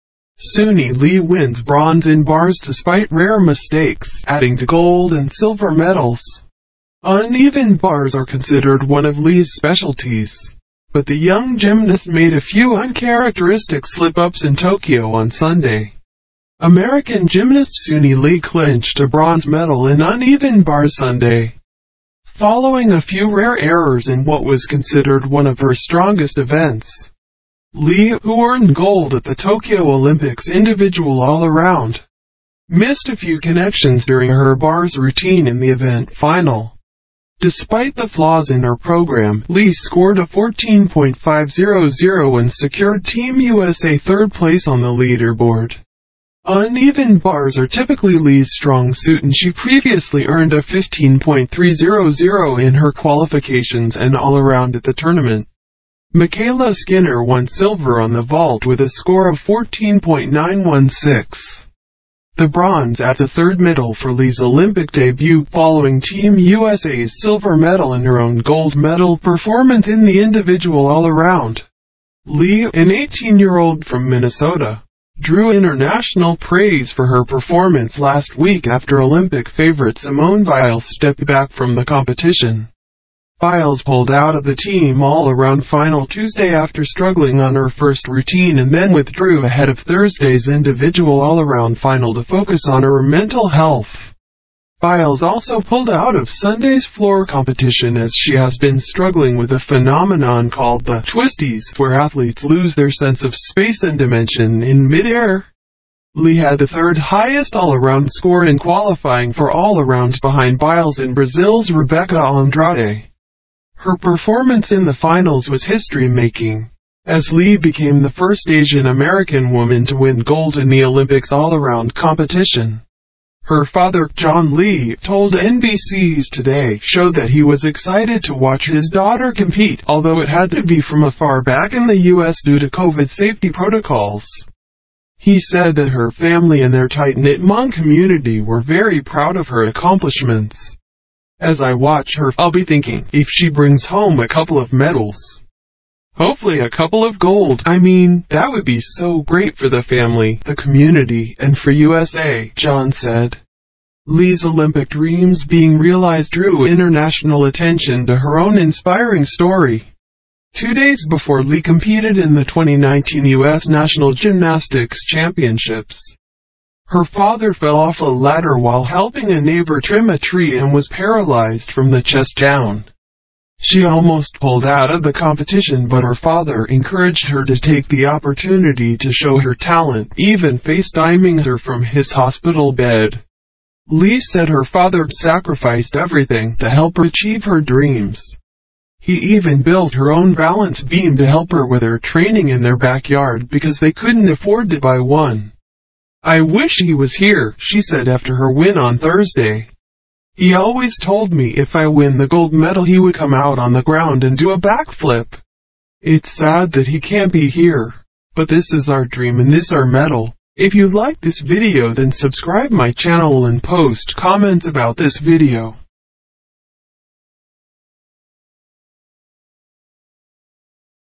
Suni Lee wins bronze in bars despite rare mistakes, adding to gold and silver medals.
Nina Derwael of Belgium took advantage of her rivals’ mistakes and nerves during the uneven bars final on Sunday to win the gold medal in her specialty.

Derwael, a two-time uneven bars world champion, did not perform as well on bars as she had in qualifying last week, when she finished first, but her routine was good enough — and difficult enough — for her to win. Looking as if she were gliding in the wind, she soared above, under and between the bars and was given 15.2 points for her performance. After the announcer called her the winner, Derwael wrapped herself in the Belgian flag and flashed a huge smile toward her small cheering section in the mostly empty Ariake Gymnastics Center.

Anastasiia Iliankova of Russia was second on the uneven bars, with 14.833 points, to win the silver medal. Sunisa Lee of the United States won the bronze, with 14.5 points.